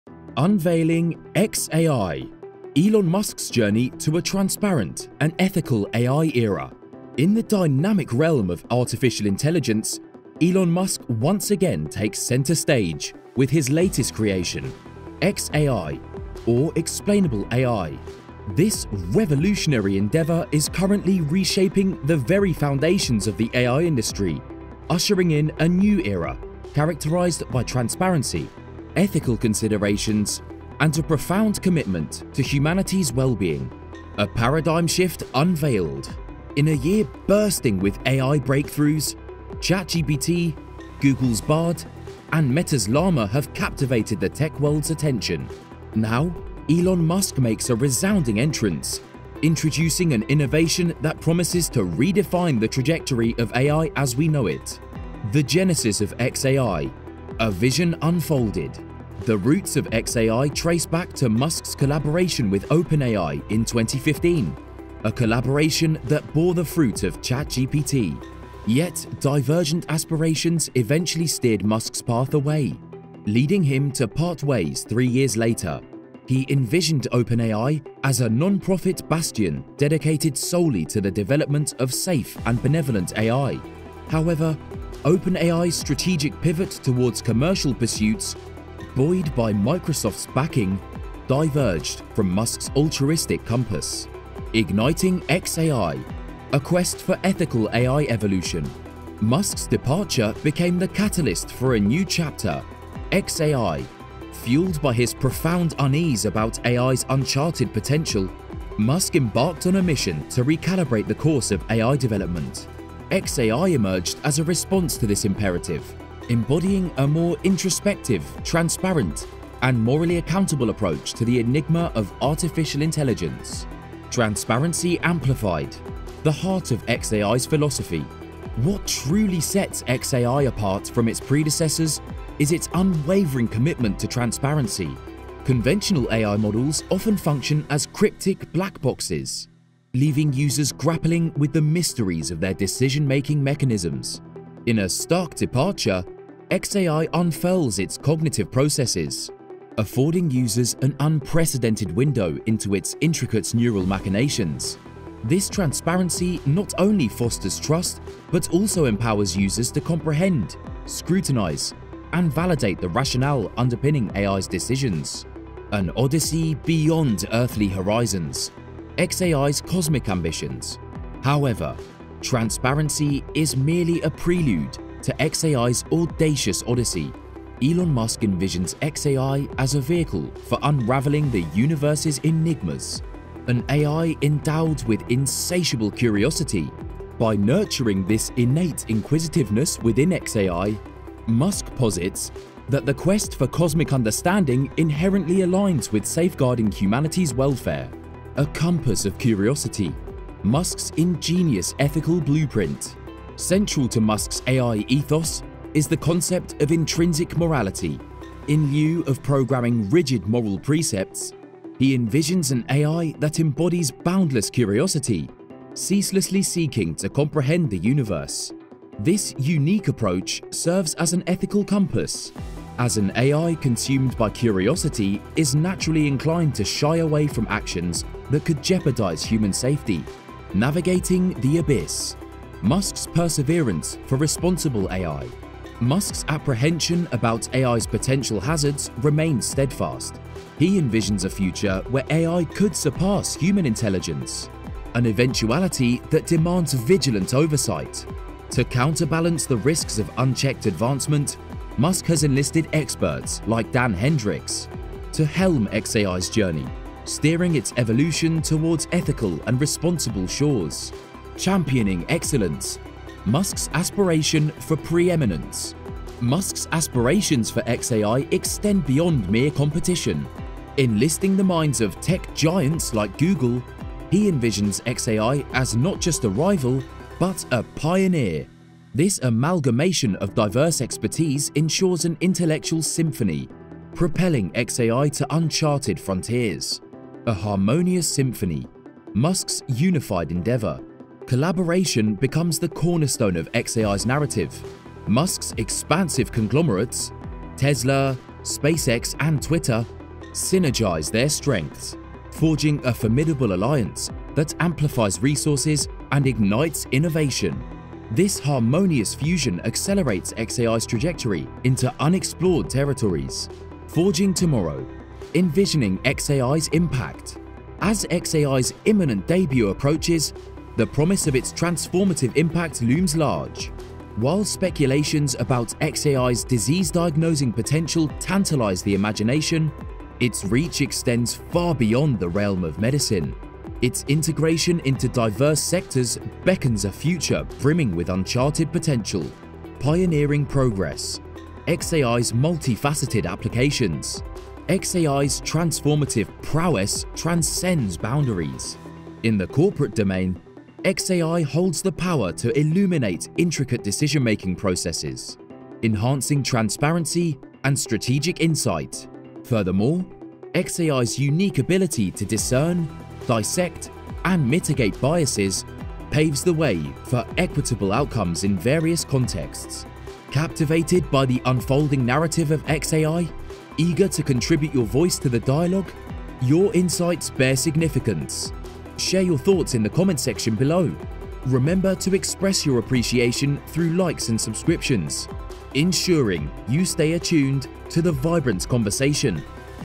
Elon Musk's Insane New xAI SHOCKS The Entire AI Industry
In the dynamic realm of artificial intelligence, Elon Musk once again takes center stage with his latest creation – XAI, or Explainable AI. This revolutionary endeavor is currently reshaping the very foundations of the AI industry, ushering in a new era characterized by transparency, ethical considerations, and a profound commitment to humanity's well-being.

Thank you for choosing AI Talks!